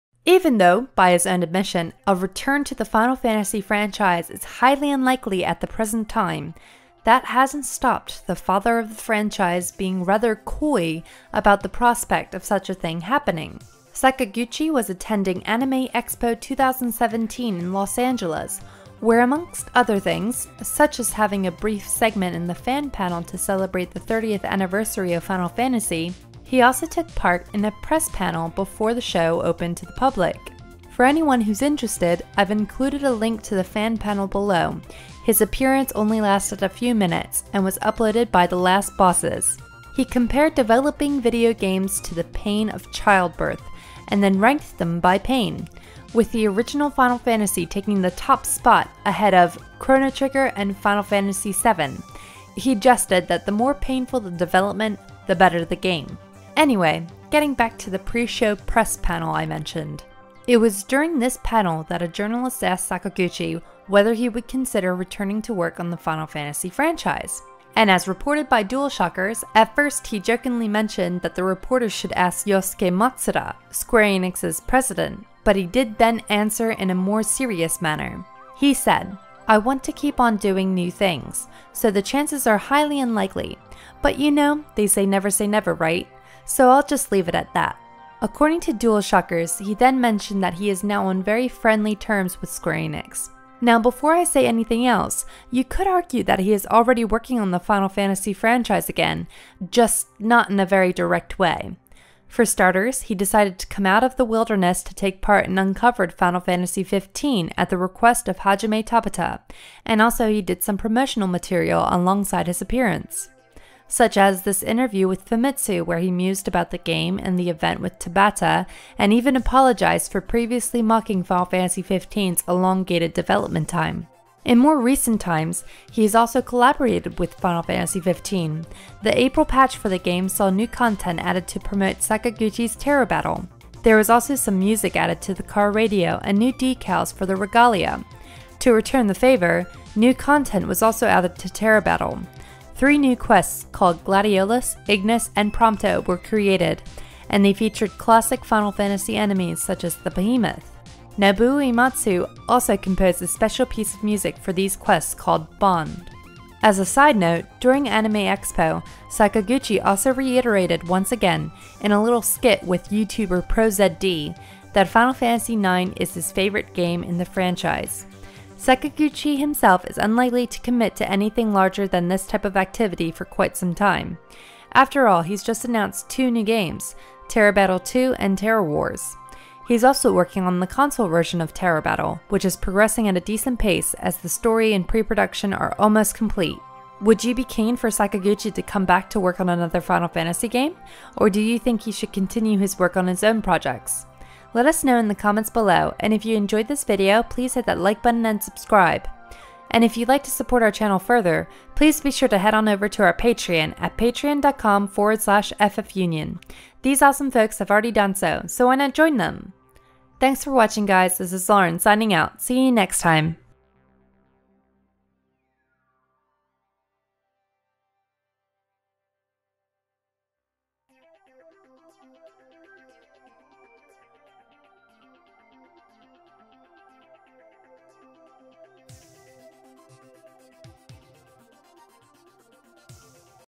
"Never Say Never": Sakaguchi Comments On Return To Final Fantasy
It will help us out a bunch as a channel. :)

During a panel at Anime Expo 2017, Hironobu Sakaguchi, the father of the Final Fantasy franchise has commented on whether or not he's likely to return to the franchise again in the future.

He also took part in a Final Fantasy 30th Anniversary panel, did a load of signing sessions and talked again (briefly) about his favourite Final Fantasy game!